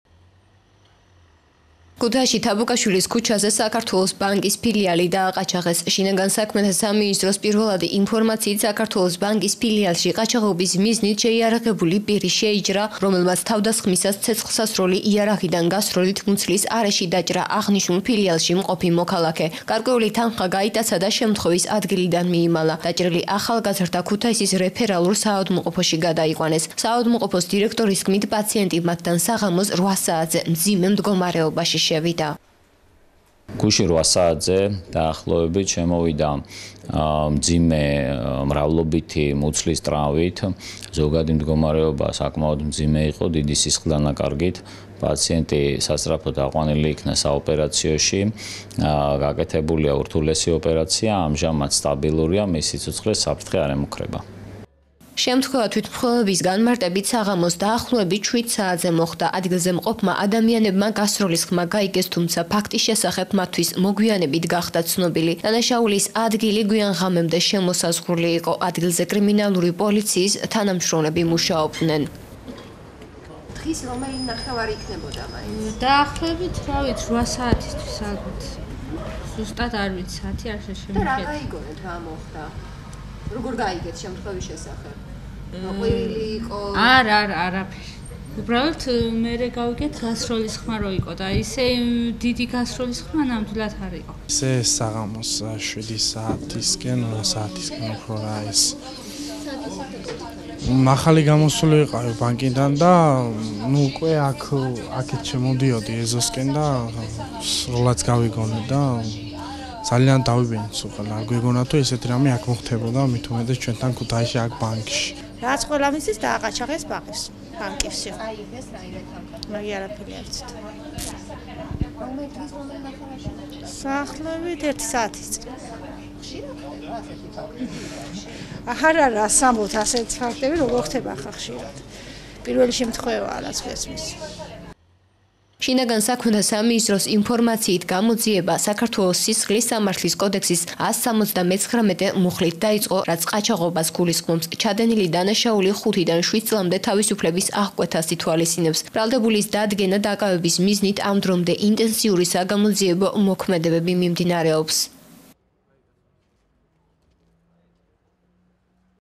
ყაჩაღობა ქუთაისში
ქუთაისში თაბუკაშვილის ქუჩაზე საქართველოს ბანკის ფილიალი დააყაჩაღეს.შინაგან საქმეთა სამინისტროს პირველადი ინფორმაციით, „საქართველოს ბანკის“ ფილიალში, ყაჩაღობის მიზნით  შეიარაღებული პირი შეიჭრა, რომელმაც თავდასხმისას ცეცხლსასროლი იარაღიდან გასროლით მუცლის არეში დაჭრა აღნიშნულ ფილიალში მყოფი მოქალაქე, გარკვეული თანხა გაიტაცა და შემთხვევის ადგილიდან მიიმალა. დაჭრილი ახალგაზრდა გადაყვანილია სამედიცინო დაწესებულებაში.  ქუთაისის რეფერალური საავადმყოფოს დირექტორის თქმით პაციენტი მათთან საღამოს 8 საათზე მძიმე მდგომარეობაში შევიდა